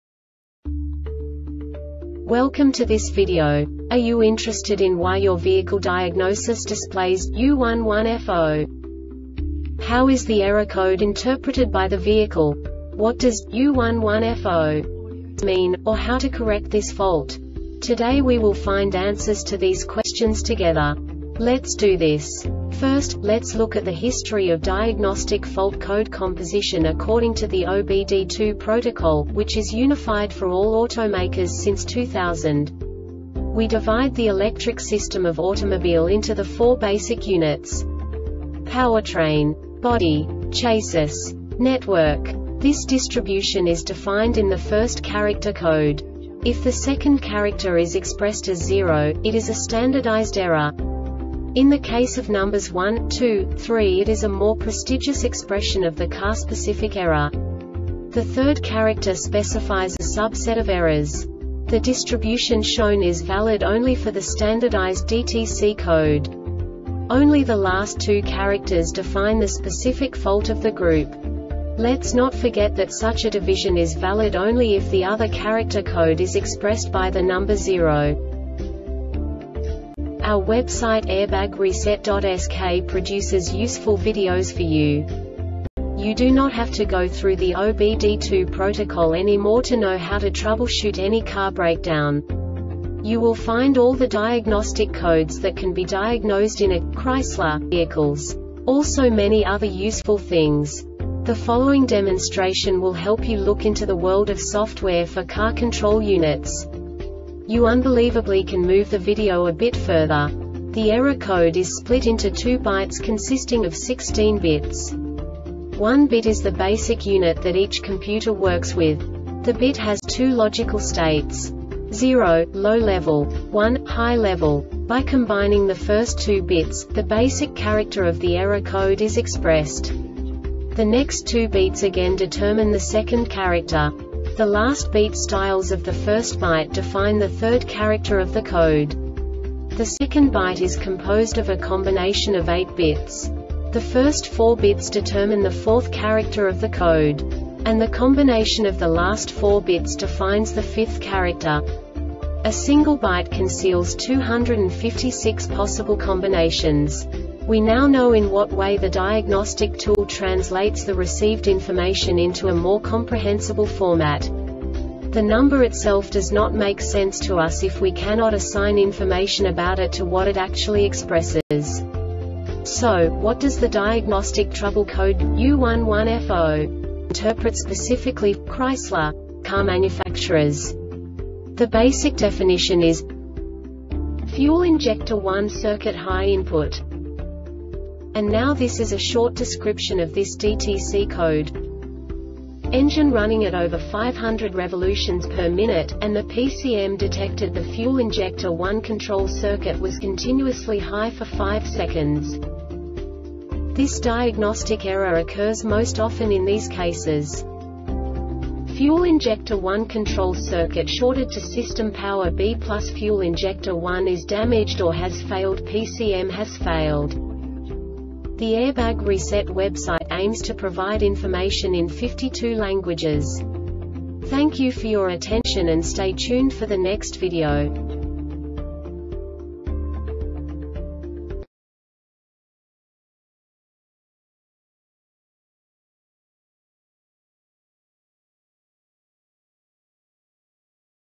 DTC Chrysler U11F0 Short Explanation
Contents:
0:21 Basic DTC analysis according to OBD2 protocol standard.
1:48 Insight into programming
2:58 A diagnostically understandable description of the Diagnostic Trouble Code
3:05 Basic error definition

Make:
Chrysler
Code:
U11F0
Definition:
Fuel Injector 1 Circuit High Input
Description:
Engine running at over 500 rpm,and the PCM detected the fuel injector 1 control circuit was continuously high for 5 seconds.
Cause:
Fuel injector 1 control circuit shorted to system power (B+)Fuel injector 1 is damaged or has failedPCM has failed